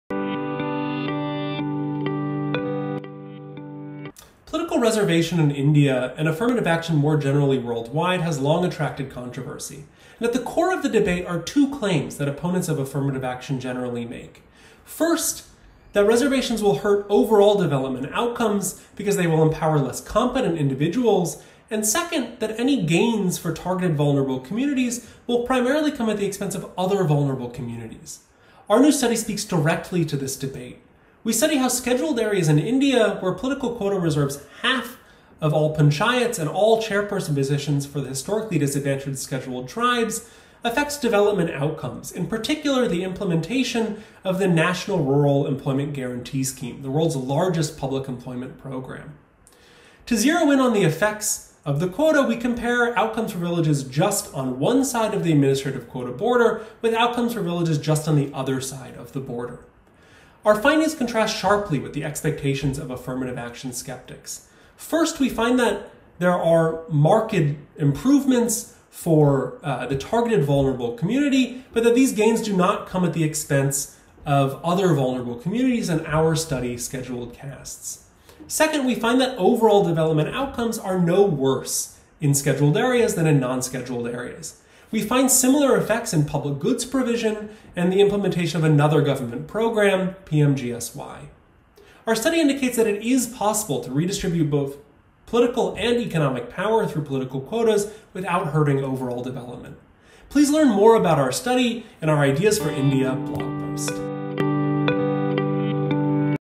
Does political reservation work, and for whom? - Nicholas Haas (Aarhus University)
Does political reservation undermine or promote development, and for whom? This
article presents an analysis of India’s Scheduled Areas, which reserve political office
for the historically disadvantaged Scheduled Tribes. Focusing on the effects on
MNREGA, it shows that reservation delivers no worse outcomes overall. There are
large gains for targeted minorities, which come at the cost of the relatively privileged
rather than other minorities.